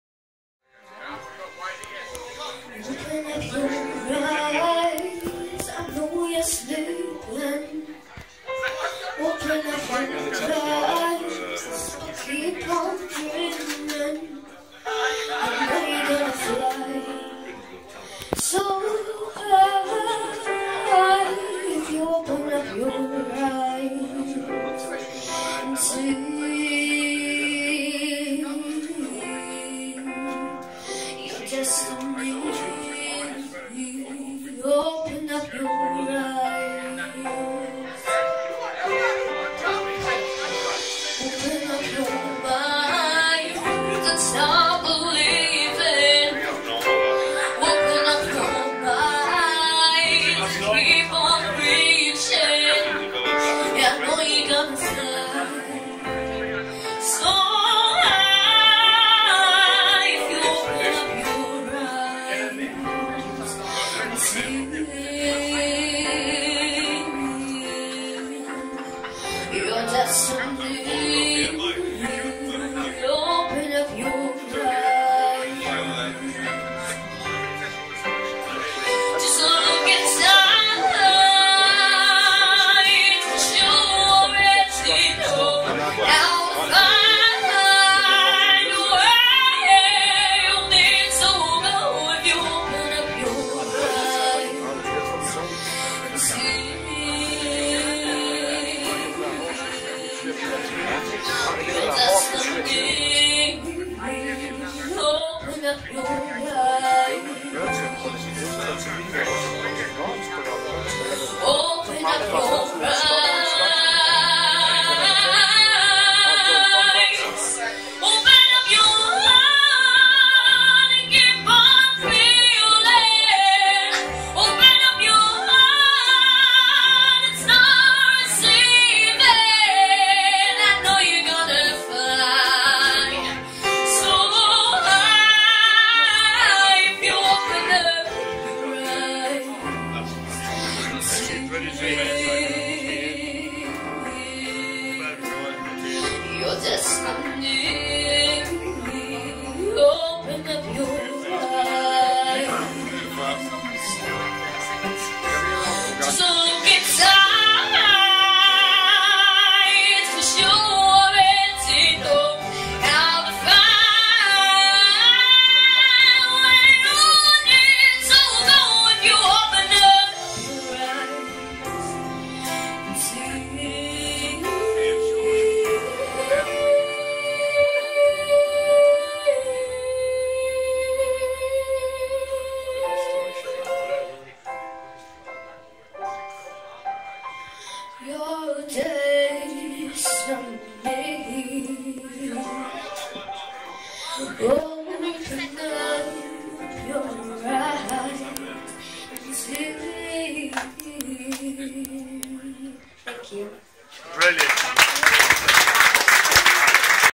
Courts .
Dun cow Friday buskers up gets  the most brilliant 11 year old named courts . Her own song as well .  (video uploaded with her parents permission)